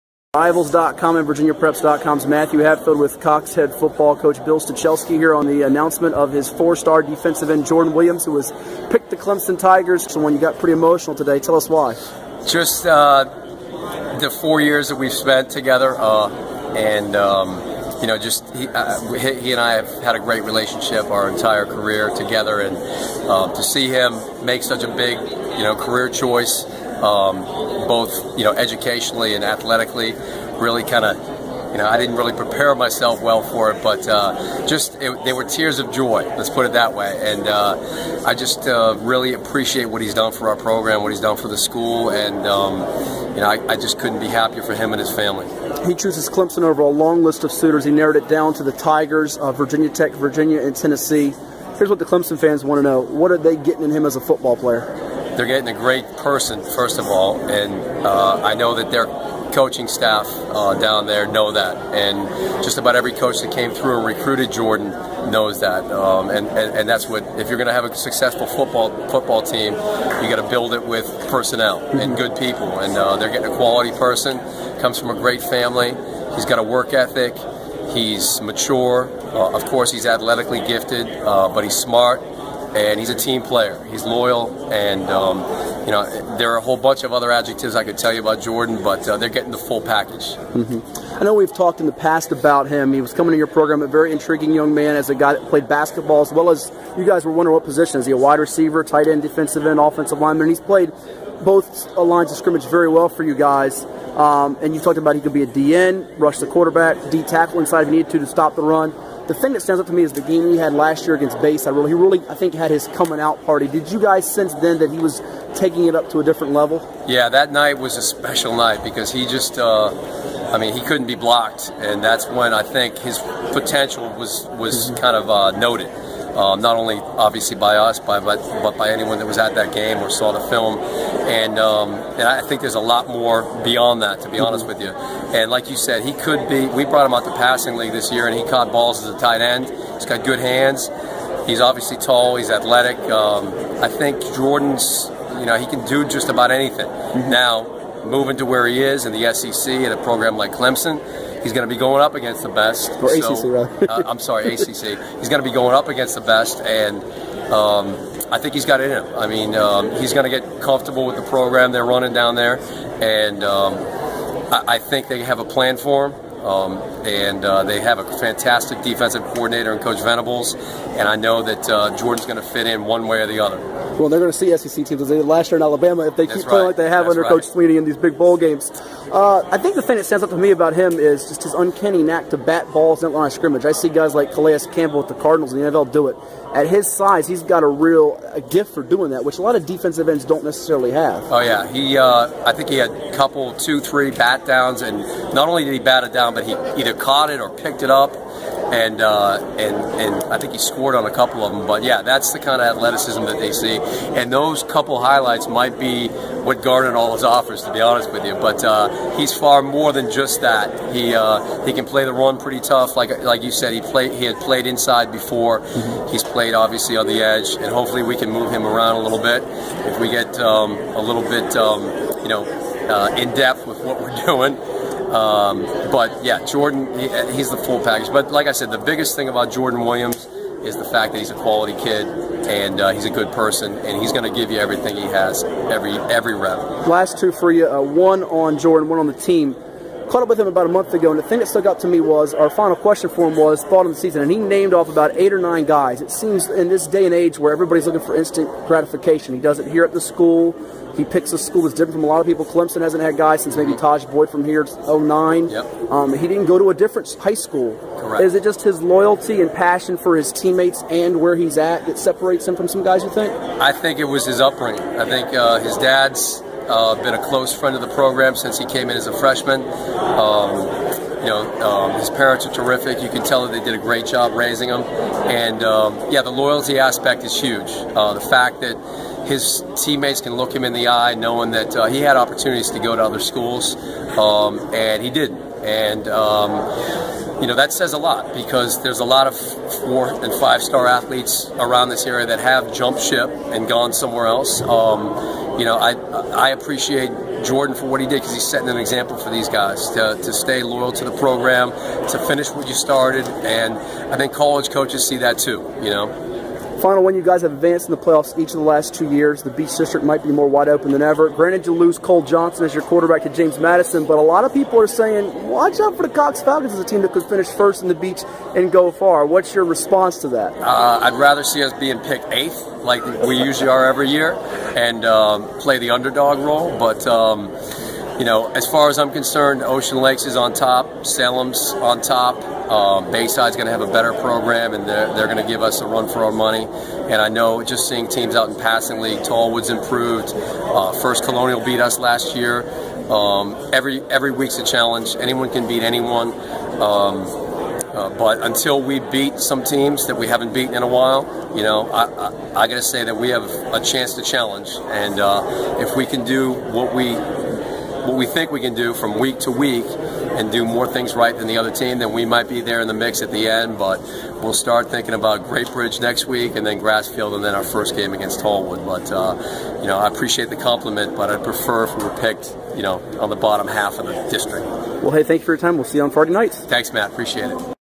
Cox Bill Stachelski on Jordan Williams to Clemson
Cox Head Football Coach Bill Stachelski talks about 4-Star DE Jordan Williams committing to play his College Football at Clemson in this Video Interview.